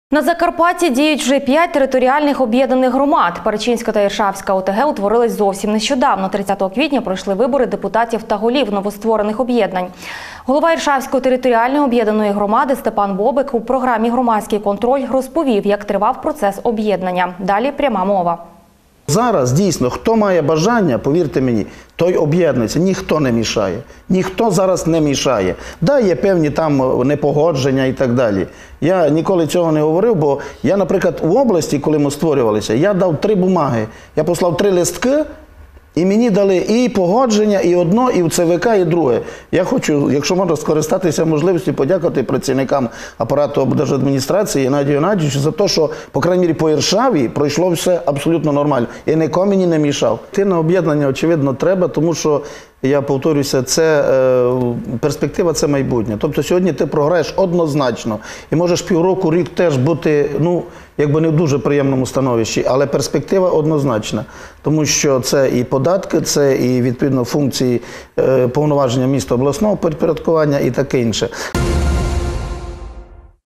Питання об’єднання громад Закарпаття
На Закарпатті діють вже 5 територіальних об’єднаних громад. Перечинська та Іршавська ОТГ утворились зовсім нещодавно. 30 квітня пройшли вибори депутатів та голів новостворених об’єднань. Голова Іршавської ОТГ Степан Бобик закликав і інших створювати об’єднані громади, адже, каже, ніхто цьому не заважає. Степан Бобик вдячний апарату ОДА , зокрема голові за оперативне вирішення питань, пов’язаних з об’єднанням. О’бєднання – це однозначно перспектива для розвитку.